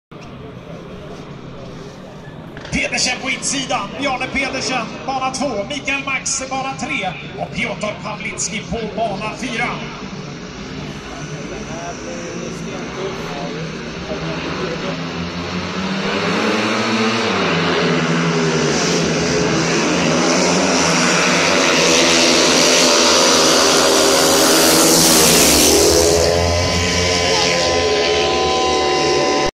Speedwaymatch Piraterna-Dackarna 20150616 Krasch Bjarne Pedersen
Speedwaymatch mellan Piraterna-Dackarna i Motala. Bjarne Pedersen krashar o kör in i Piotr Pawlickis cykel.